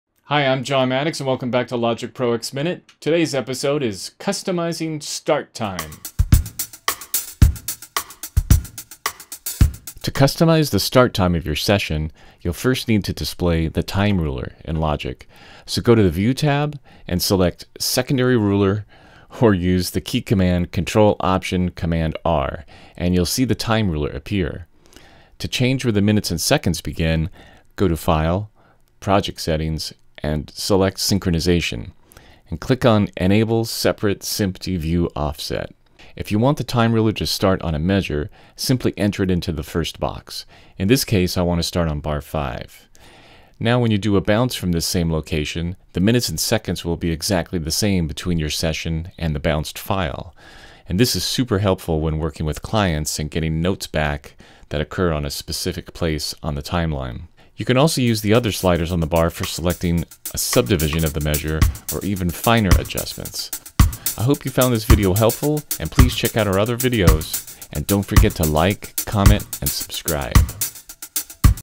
Logic Pro X Minute: Customizing Start Time
Tell Logic where you want the minutes and seconds to begin in your Logic session.